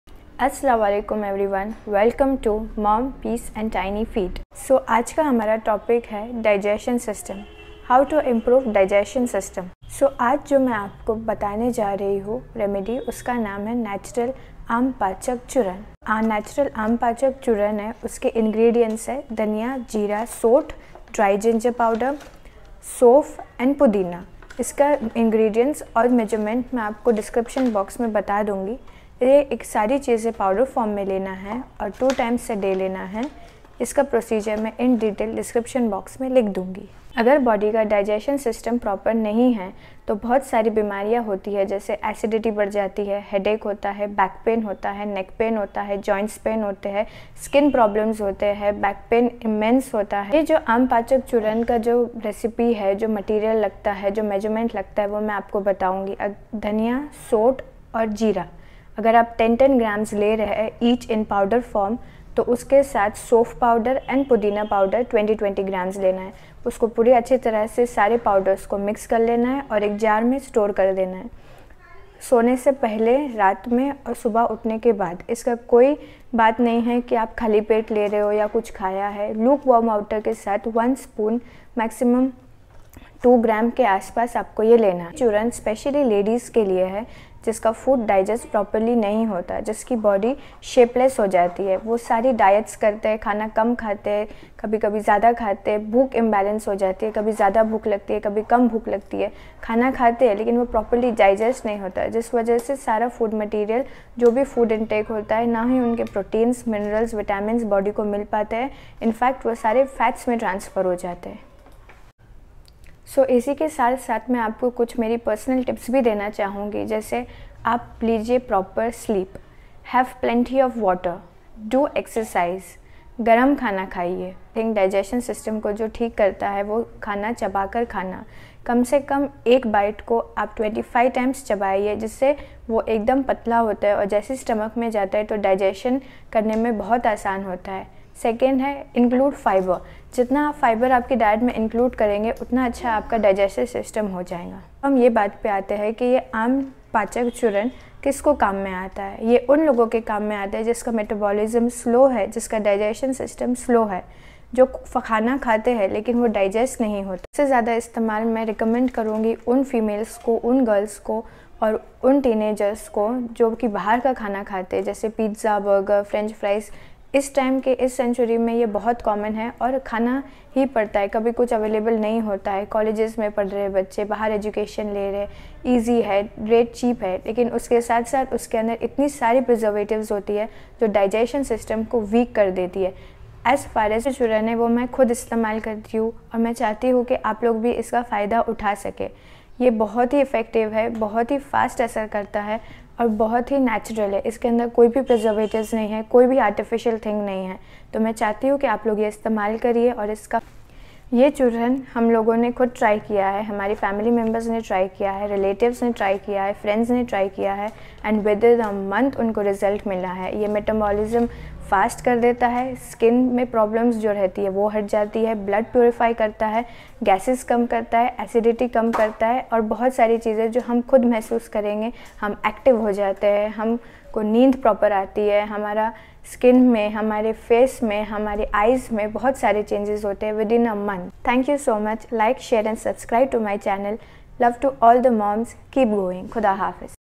LOSE WEIGHT And FIGHT OBESITY MOMBLOGGER AAMPACHAKCHURN#NATURAAL
Aam pachak churn
Dhaniya powder (coriander powder )10grms
Soth powder(dry ginger) 10 grms
Jeera powder (cumin powder) 10 grms
Sofe powder(fennel powder ) 20 grms
Pudina powder (mint powder) 20 grms
Mix it all well
Have it with luke warm water
Before and after bed

Lots of love to all the womens out there ✌️✌️✌️✌️